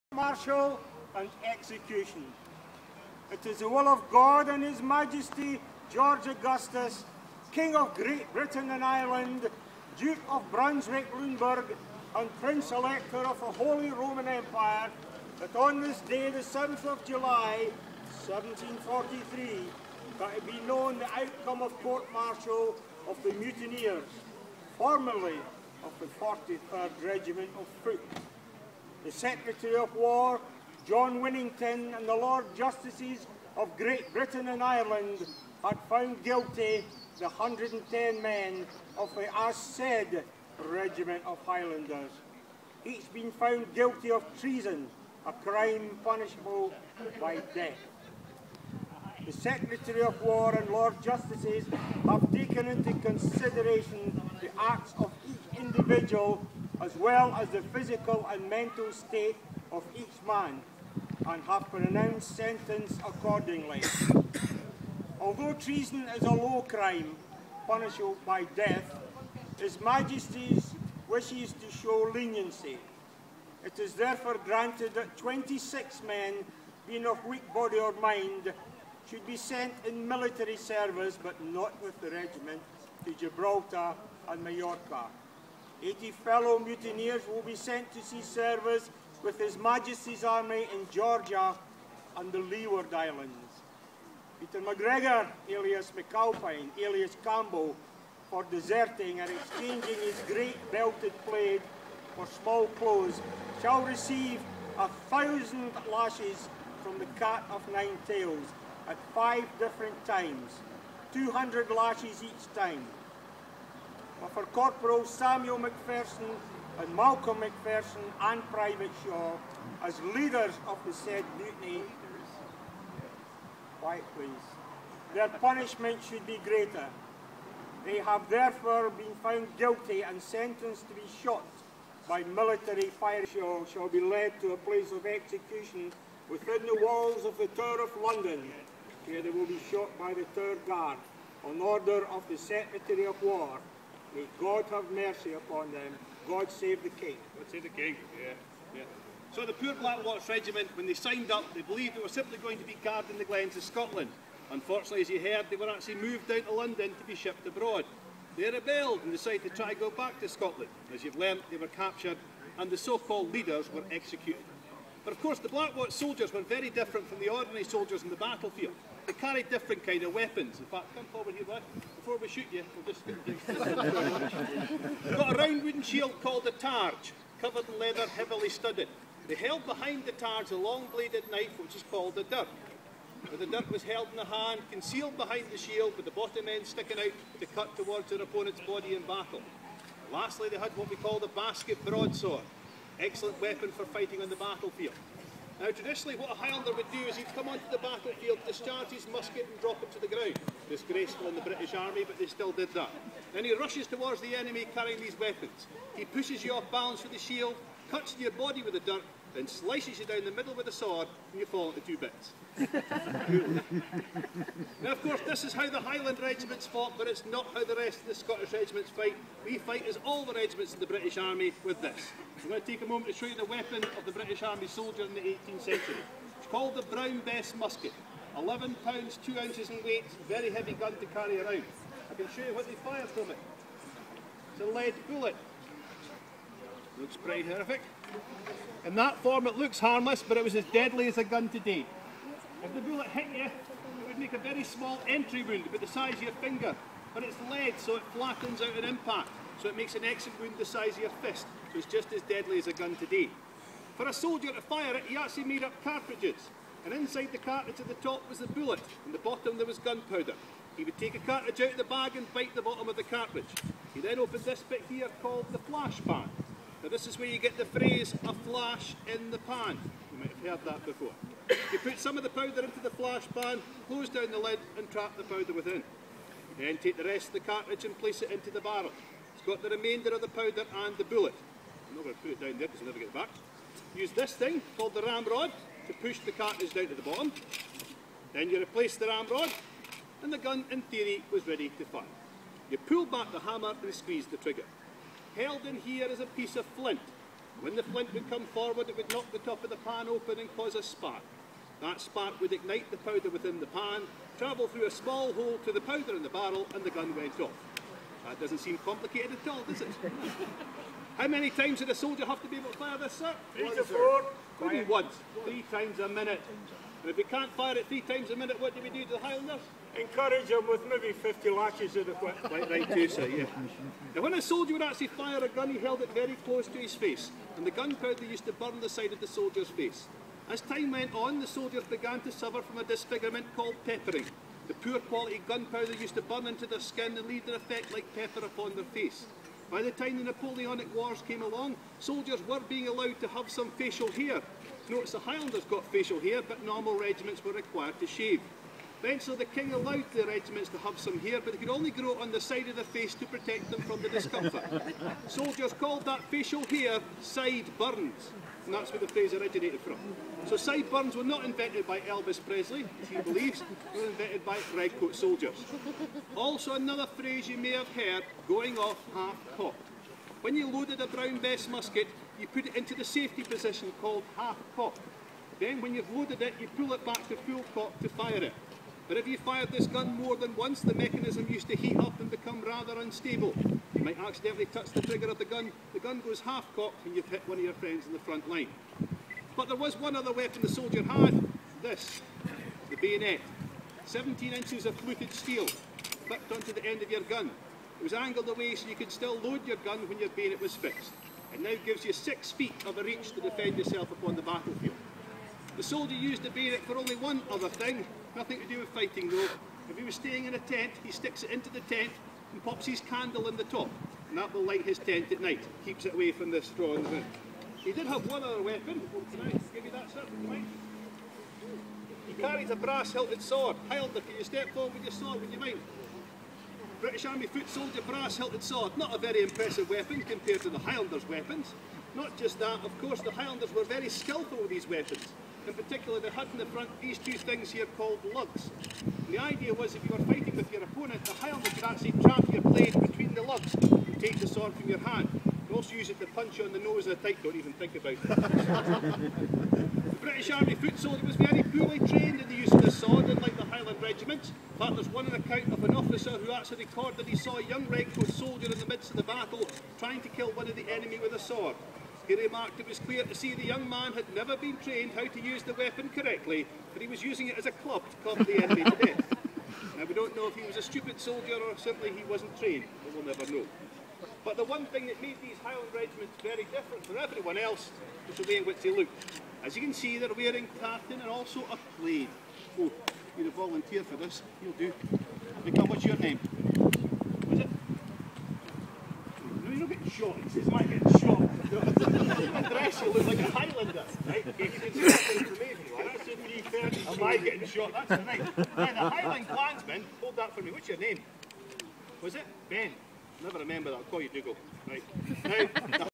1743 Mutiny re-enactors at Black Watch Museum for Festival of Museums.
1743 Mutiny re-enactors at Black Watch Museum for Festival of Museums.

The red coat reads out the judgement on the mutineers, while his colleague is eager to start the executions! Later, one of the soldiers talks about the difference between Highland and Lowland weaponry, and shows how a Highland soldier would have used his weapons.